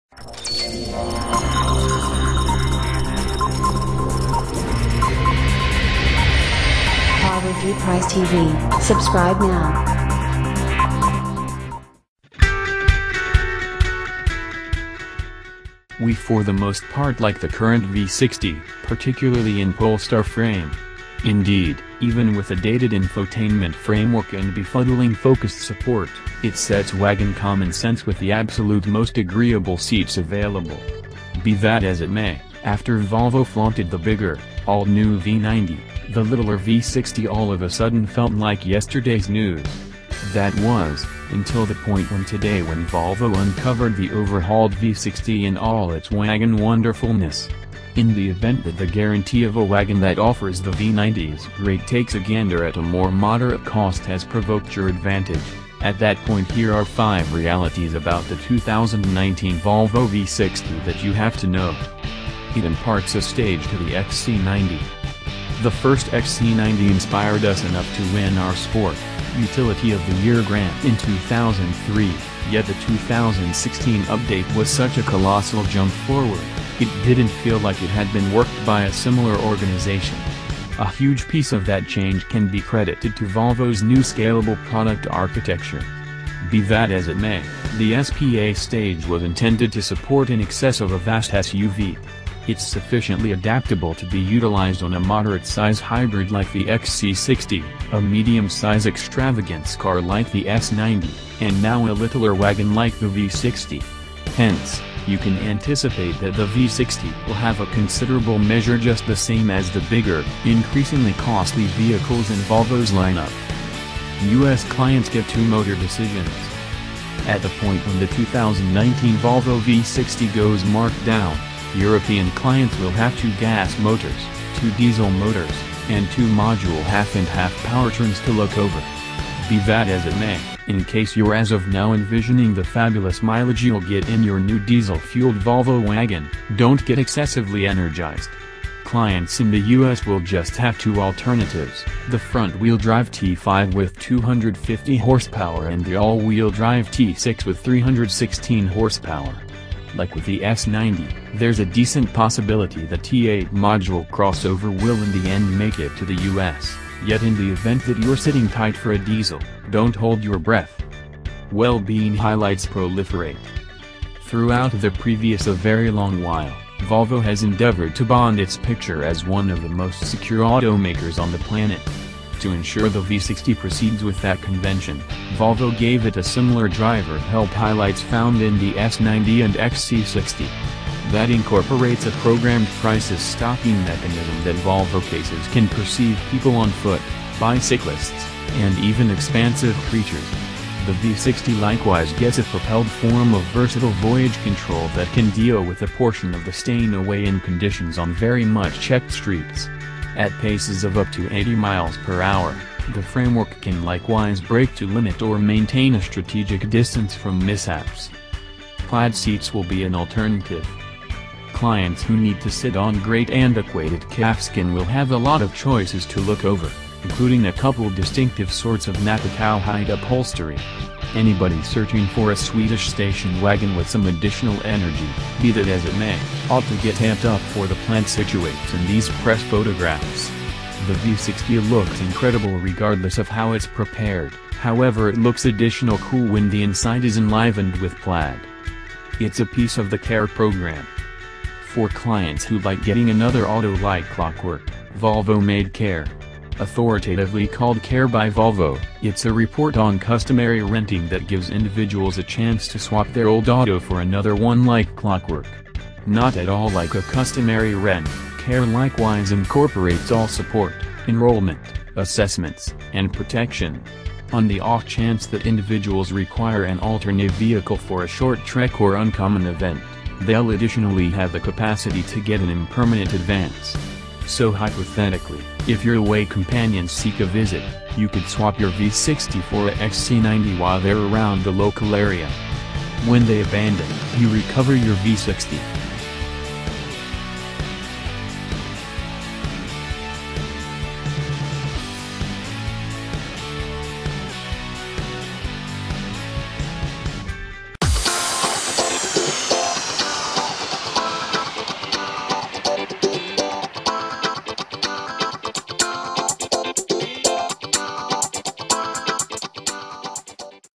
2019 Volvo V60 Five Things You Need To Know
We for the most part like the current V60, particularly in Polestar frame. Indeed, even with a dated infotainment framework and befuddling focus support, it sets wagon common sense with the absolute most agreeable seats available. Be that as it may, after Volvo flaunted the bigger, all-new V90, the littler V60 all of a sudden felt like yesterday's news. That was, until the point when today when Volvo uncovered the overhauled V60 in all its wagon wonderfulness. In the event that the guarantee of a wagon that offers the V90's great takes a gander at a more moderate cost has provoked your advantage, at that point here are five realities about the 2019 Volvo V60 that you have to know.

It imparts a stage to the XC90.

The first XC90 inspired us enough to win our Sport/Utility of the Year grant in 2003, yet the 2016 update was such a colossal jump forward, it didn't feel like it had been worked by a similar organization. A huge piece of that change can be credited to Volvo's new Scalable Product Architecture. Be that as it may, the SPA stage was intended to support in excess of a vast SUV. It's sufficiently adaptable to be utilized on a moderate size hybrid like the XC60, a medium size extravagance car like the S90, and now a littler wagon like the V60. Hence, you can anticipate that the V60 will have a considerable measure just the same as the bigger, increasingly costly vehicles in Volvo's lineup.

U.S. clients get two motor decisions. carreviewpricetv Car Review Price TV